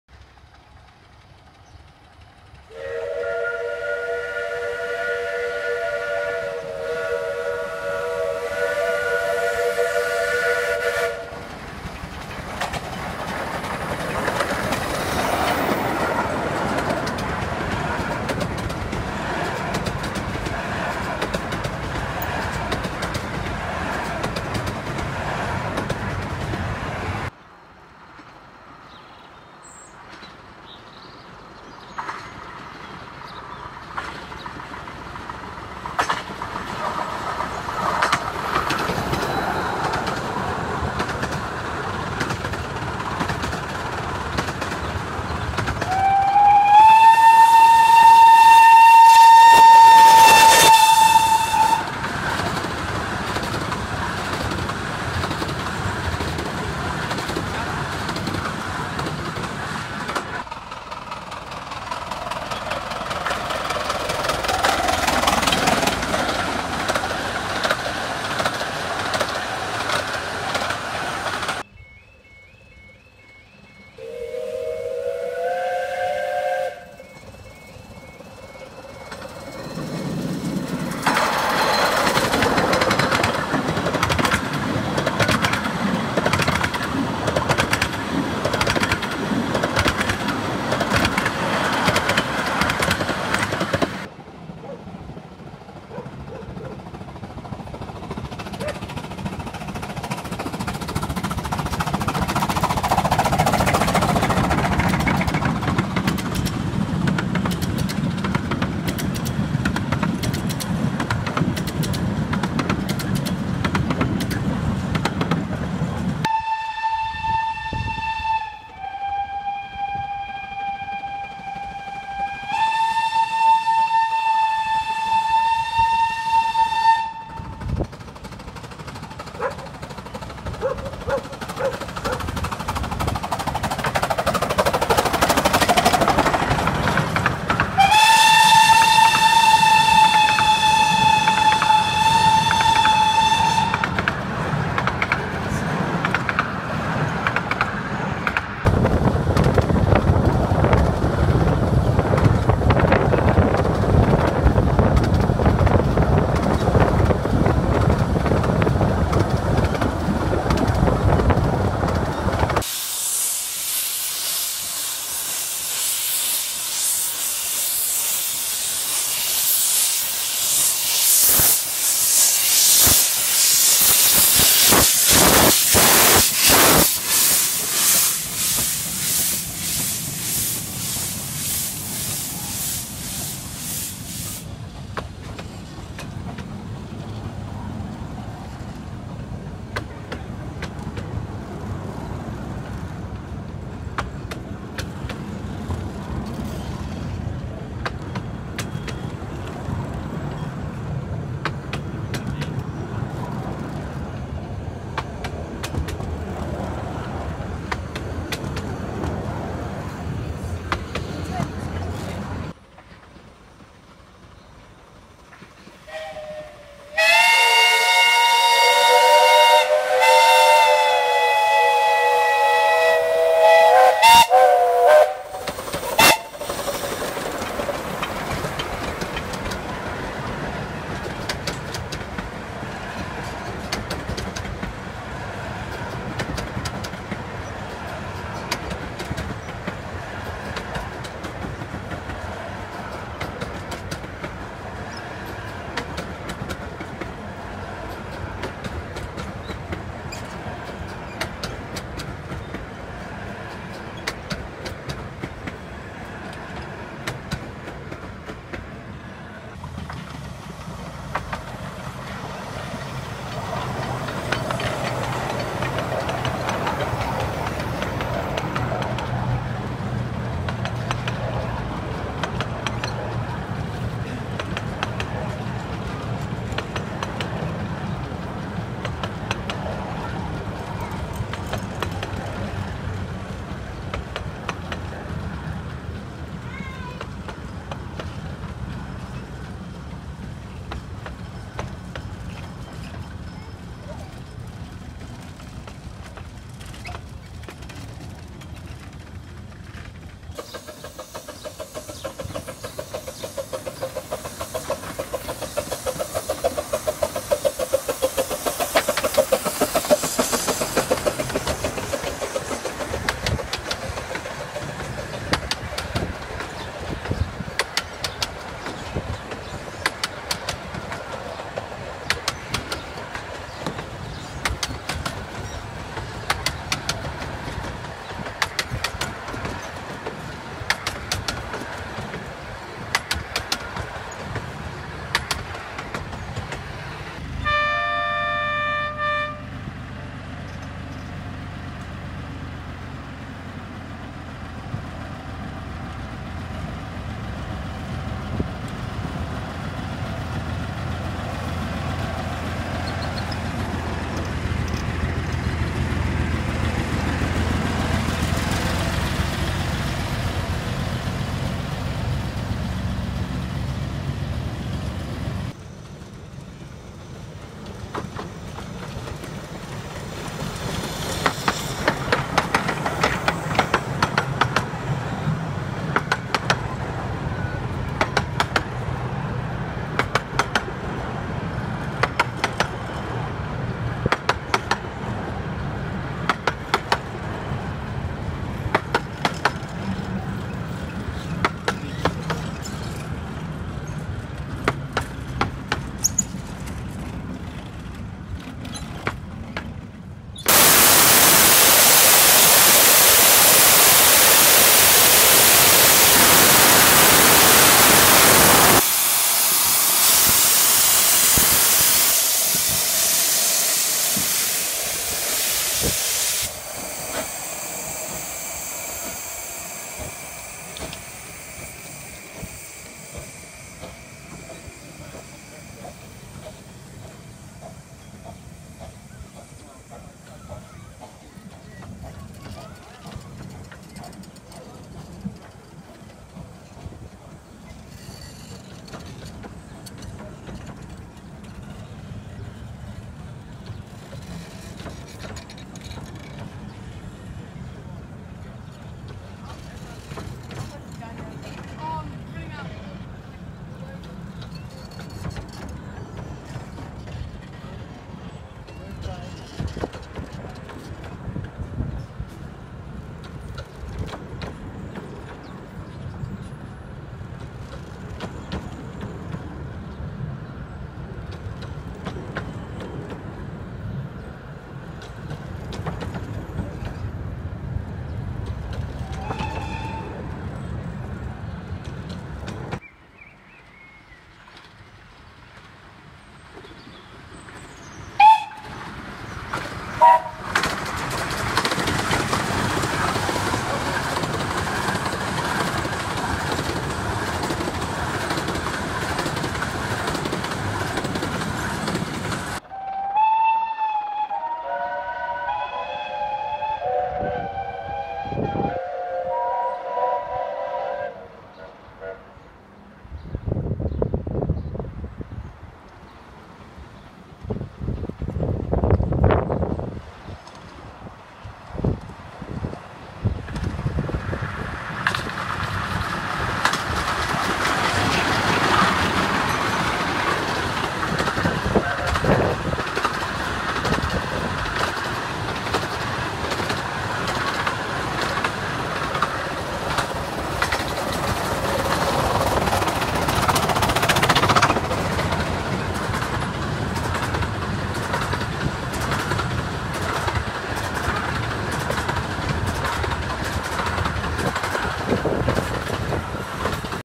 Romney, Hythe & Dymchurch Railway Steam & Diesel Gala 2019
Romney, Hythe & Dymchurch Railway (RH&DR) Steam & Diesel Gala 2019

Visiting Engine Whillan Beck is included
-Apologies for no diesel clips - not many came.

Filmed over two days 11th and 12th May